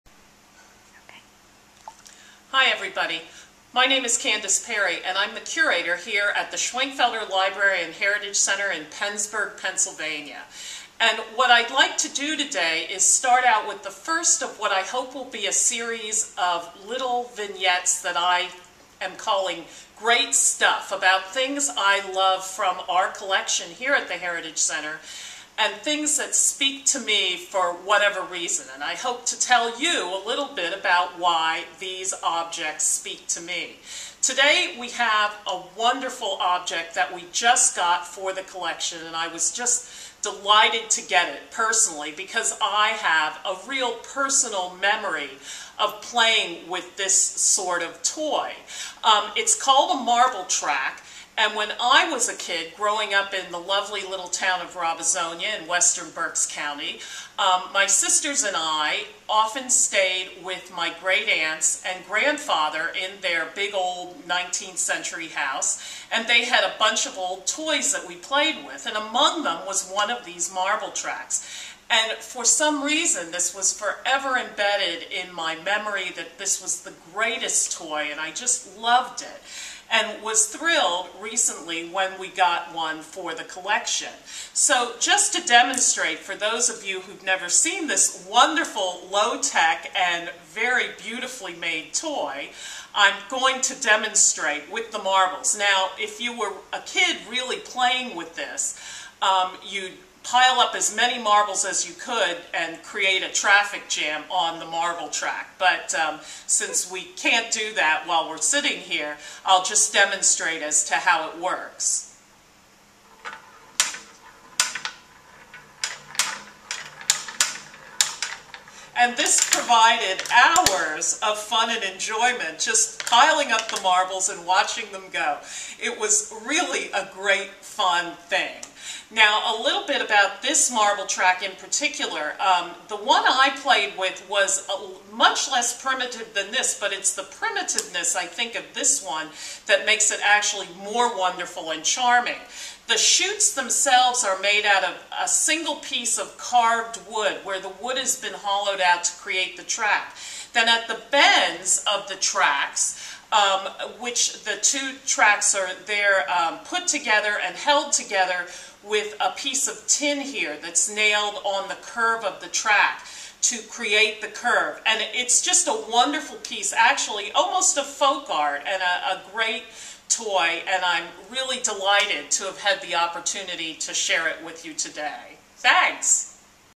MarbleTrack.avi
SLHC Curator Candace Perry talks about a marble track in the collection. This is part of a new 2011 video series of collection highlights called Great Stuff.